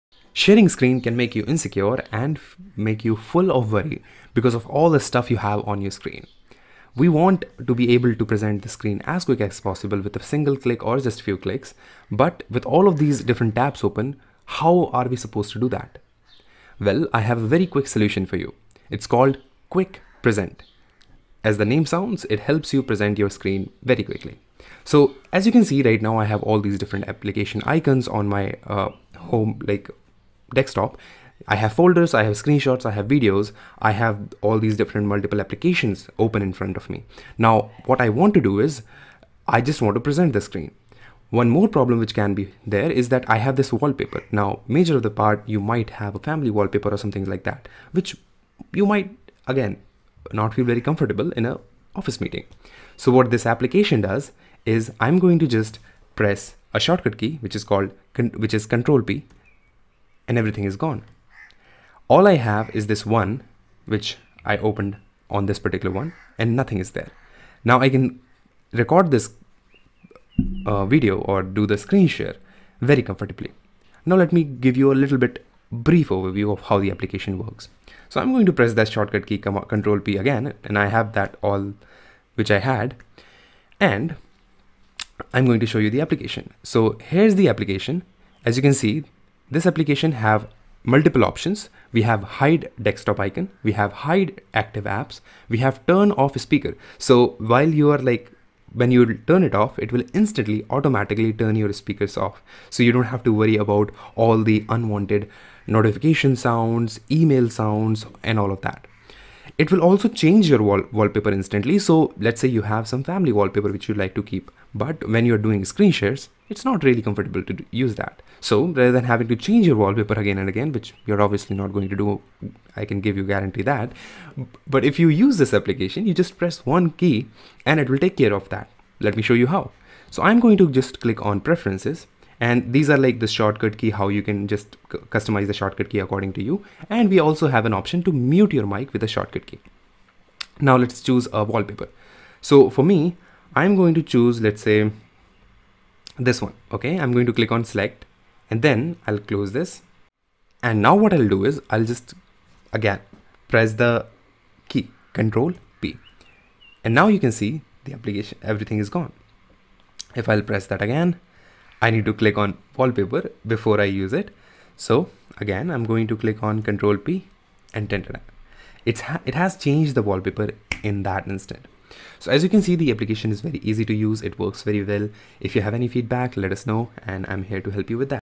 QuickPresent App Lifetime Deal $16 - Single click worry free screenshare | QuickPresent App Review
QuickPresent App is One-click to hide desktop icons, minimize active apps, turn off speakers, change wallpaper, and disable Notifications.

How To Buy QuickPresent App AppSumo Lifetime Deals @($16)
✅ Wait for some seconds, An Discount popup will appear.